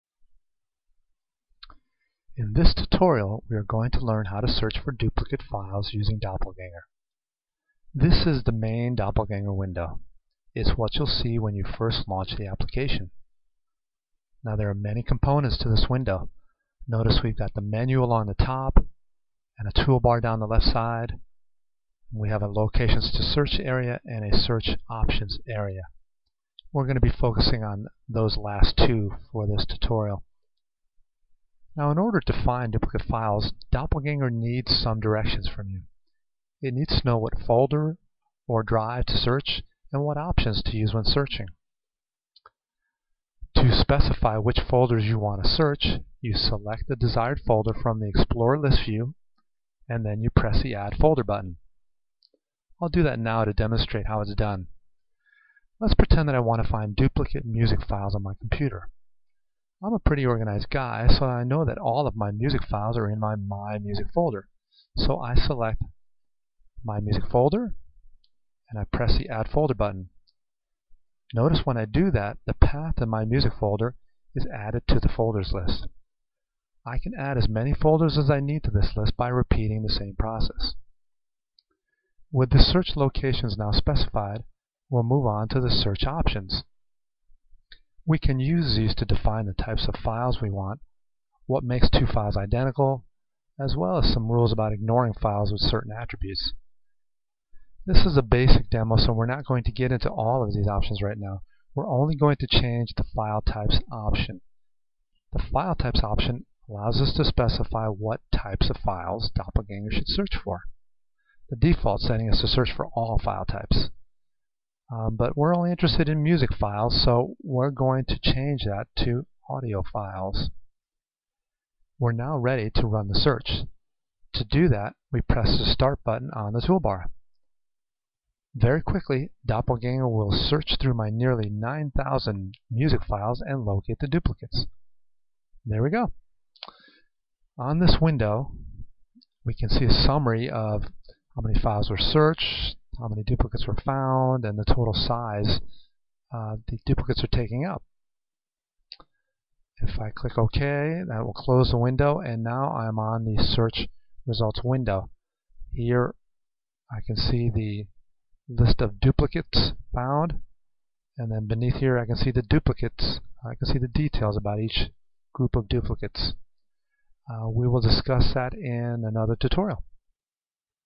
Doppelganger Tutorial 1: How To Search for Duplicate Files
How to search for duplicate files using Doppelganger.

Doppelganger, is a powerful and reliable tool to find and remove duplicate pictures, documents, music files, and more! Removing duplicates frees up disk space and aids your computer in indexing.  If you backup your files, removing duplicates first reduces the storage space required for the backups and the amount of time required to perform the backup.  Doppelganger is a Windows file management utility that can find matching files even if their names are different!